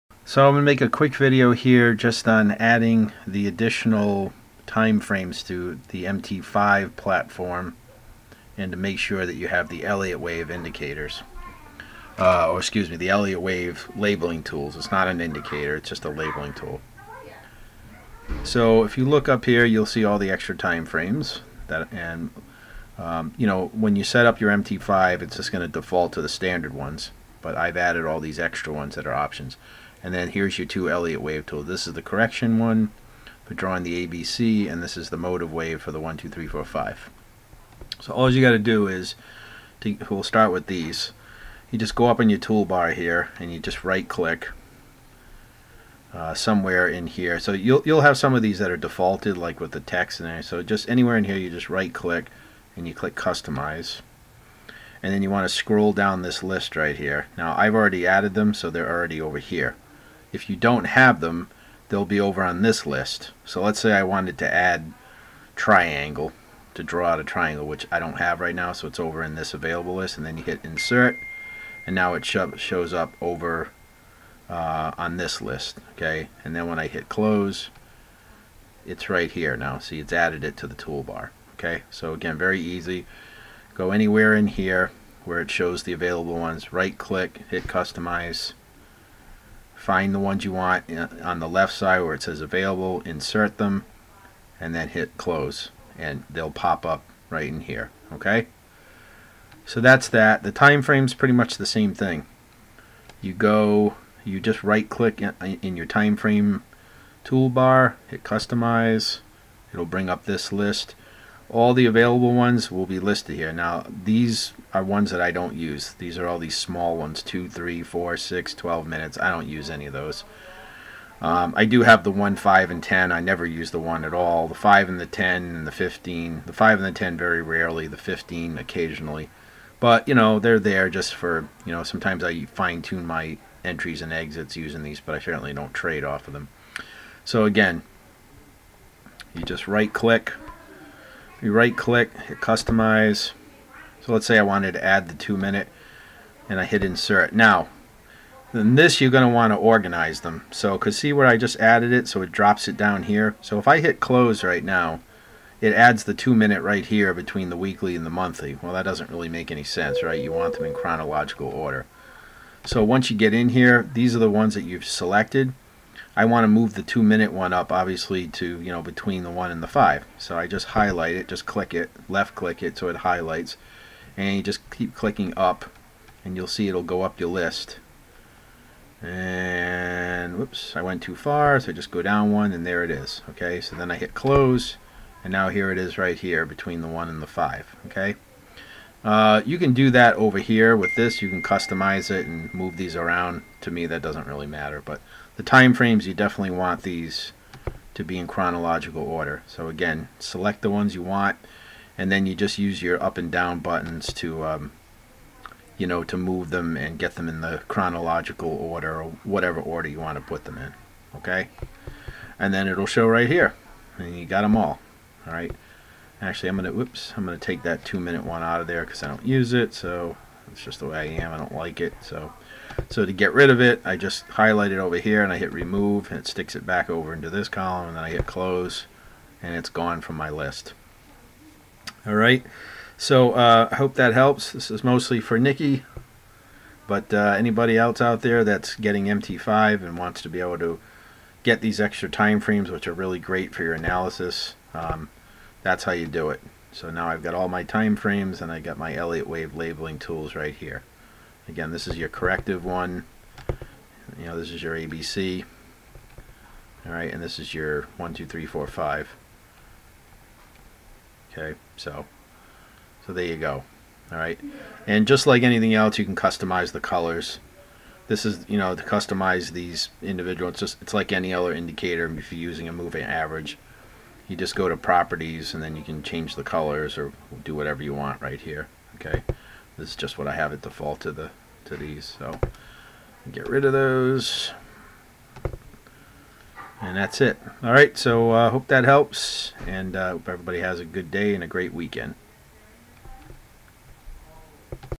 Add Extra time frames & Elliott Wave Labels to MT5
If you are an Elliott Wave trader, these are critical tools to have for analysis. These are NOT available in MT4!!